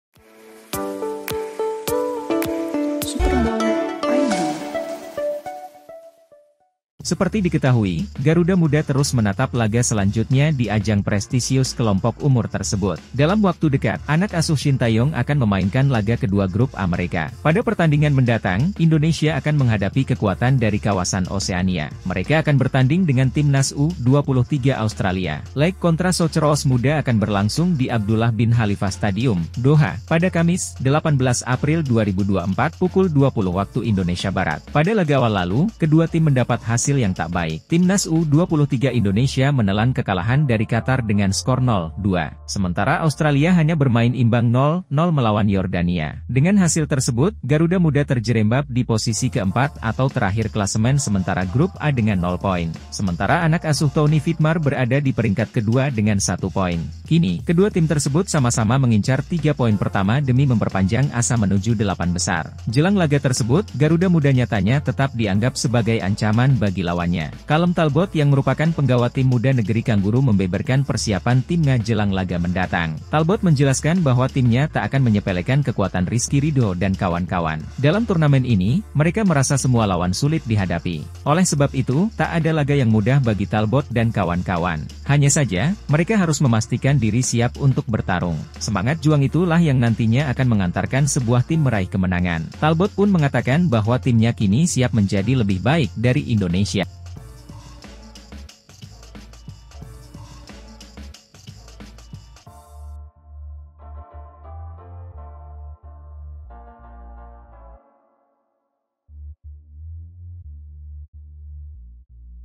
Sama sama Incar 3 Poin Pertama, Australia Haram Pandang Remeh Timnas U-23 Indonesia
SUPERBALL.ID -  Australia enggan menyepelekan kekuatan Timnas U-23 Indonesia jelang pertandingan kedua Grup A Piala Asia U-23 2024.

Artikel :  M Hadi Fathoni/Superball.id
Sumber Foto : Instagram.com/timnas.indonesia | AFC.com
Video Editor : Tommy Nicolas/Superball.id

Like & Follow Facebook:
Superball